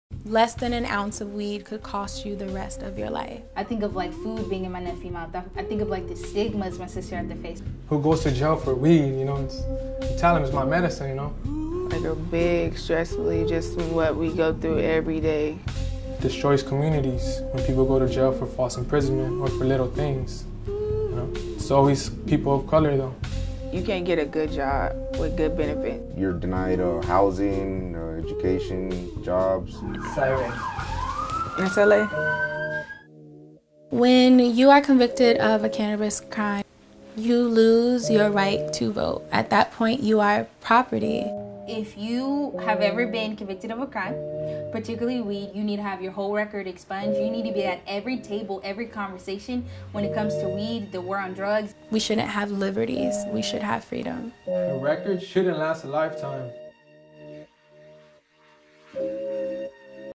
A Record Shouldn't Last A Lifetime | Besito x Equity First Alliance
In support of @expungementweek we partnered with @besito and @equityfirstalliance to create A Record Shouldn’t Last A Lifetime, a powerful campaign featuring advocates, formerly incarcerated individuals, and their family members, amplifying the voices of those impacted by the war on drugs. The stories were captured from the @equityfirstalliance community by Tree Femme creatives putting an intimate and human face to those who have been most harmed, by telling their personal stories.

The campaign is a call to action advocating for widespread implementation of automated expungement and for expanded eligibility for expungement. We are urging every state to implement speedy, accessible, and thorough processes to automatically expunge the records of all who are eligible.

To watch all 4 films and to sign our petition advocating for more access to automated expungement, click the link in our bio! Campaign produced by @treefemmecollective photo/styling by: @thaliagochez directed/edited by @_flamboient DP @andres_solorzano music @b11ce
Stories: @bigtimecannabis @tau_creative @dejanaetayne @handsup_la @gameetight_